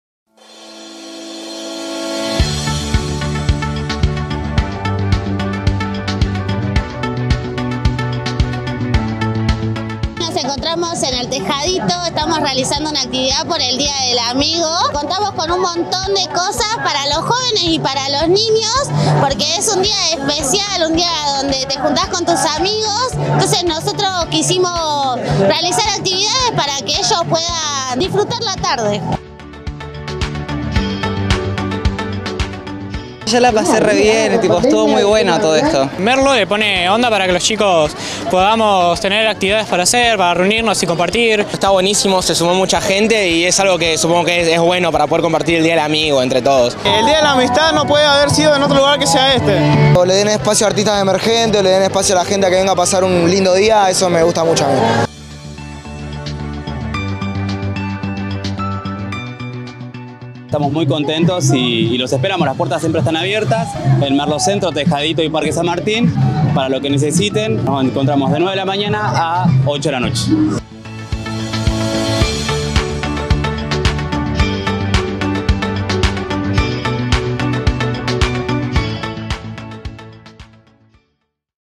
MERLO CELEBRÓ LA AMISTAD
La Subsecretaría de Juventudes de Merlo realizó una jornada especial por el Día del Amigo en el Parque Municipal Néstor Kirchner, donde una multitud de jóvenes y familias se acercaron a celebrar la amistad.